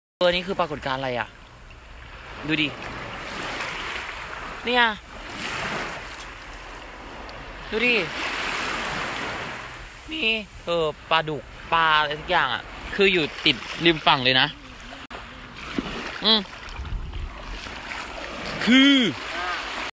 Catfish Make Strange Swimming Ball
This is the bizarre moment hundreds of fish swam together forming a strange ball in the shallow part of a sea in eastern Thailand.

Footage shows the striped eel catfish swimming closely to form the ball in Koh Chang, Trat province.